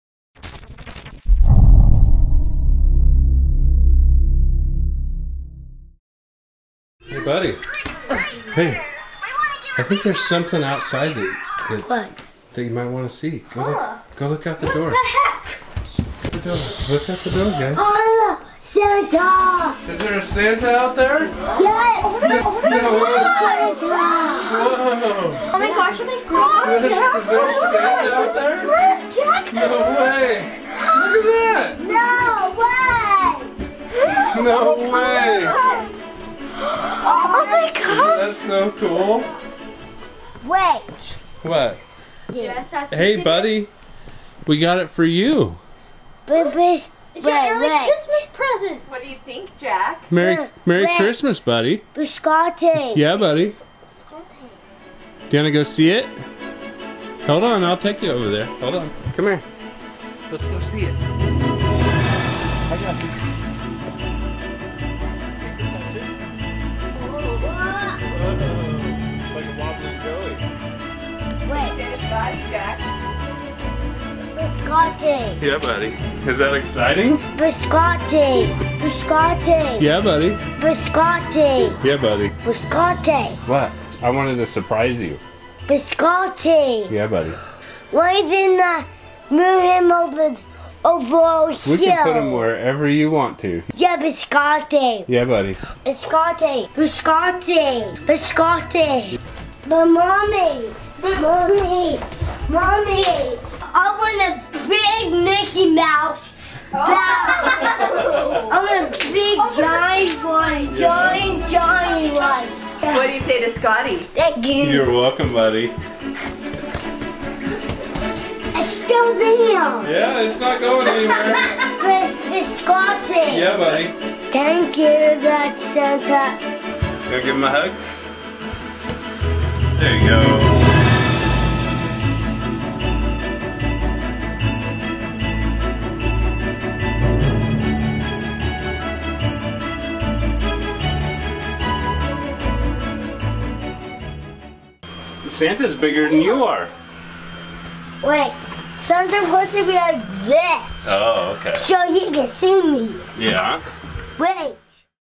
Jack and the balloon Santa Clause
We got jack a balloon Santa Clause and his reation is pricless!!